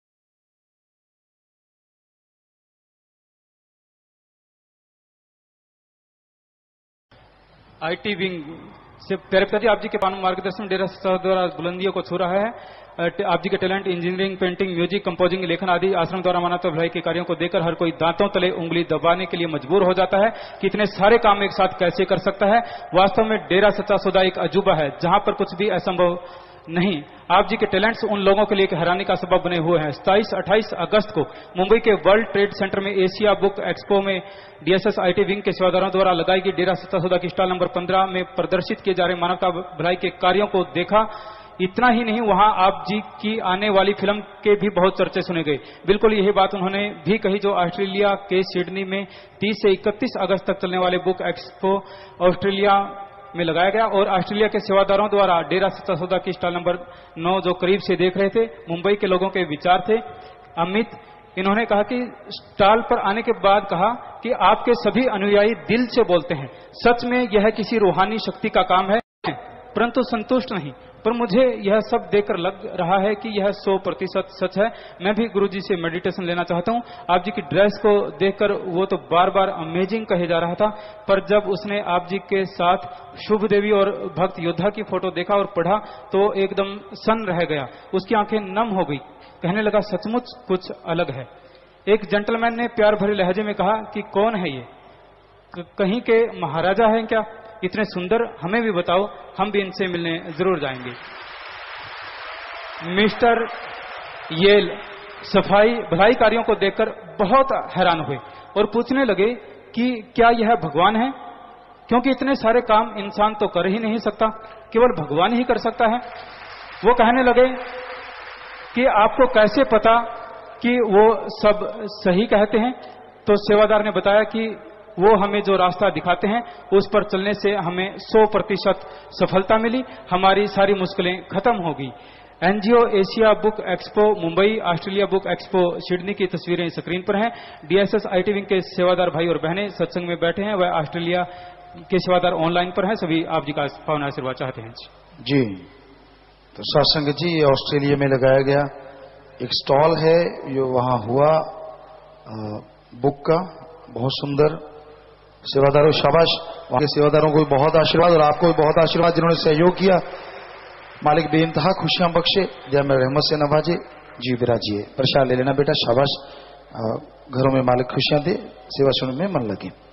Q.130 मुंबई और ऑस्ट्रेलिया बुक एक्सपो में गुरूजी की तारीफ. | Graced By Saint Gurmeet Ram Rahim Singh
Saint Dr. Gurmeet Ram Rahim Singh Ji Insan